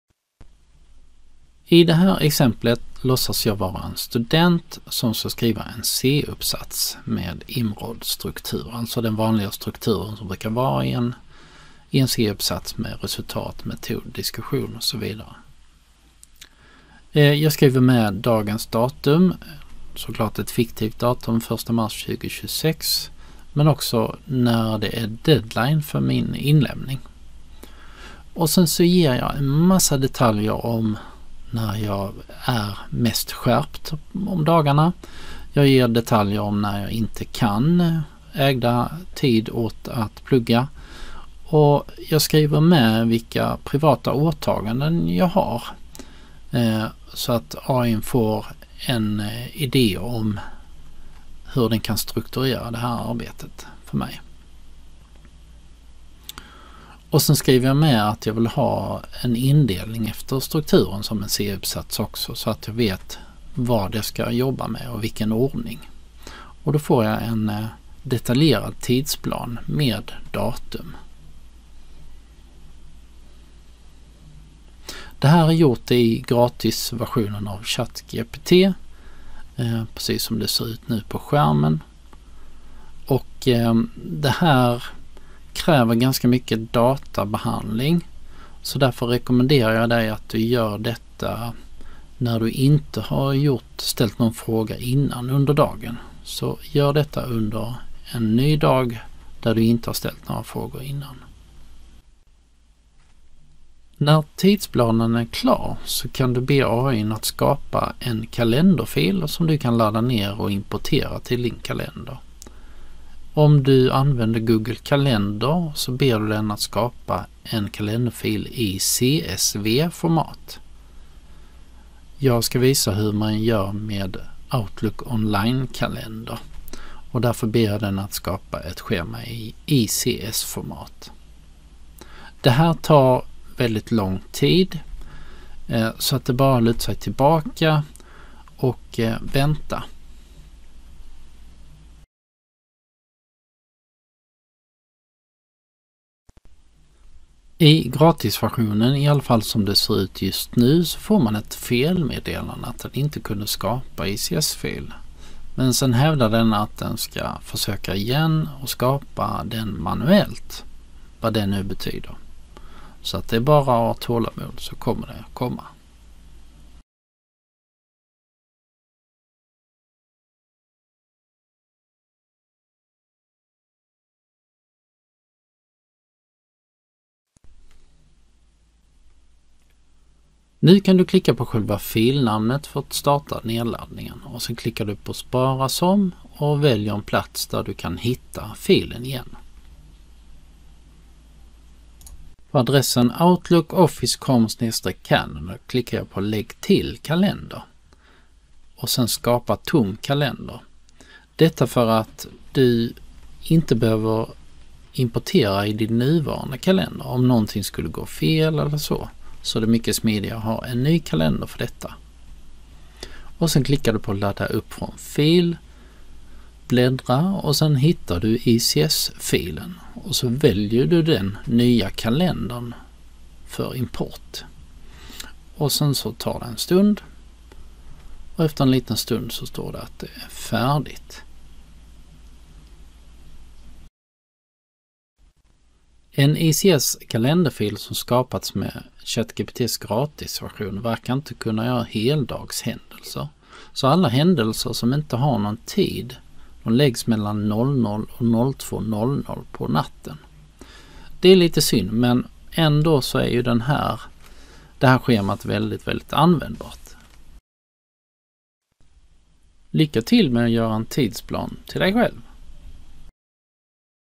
Skapa tidsplan och arbetsflöde för ditt akademiska arbete
Lärresurs om generativ AI av Dan Wirdefalk, Högskolan Kristianstad.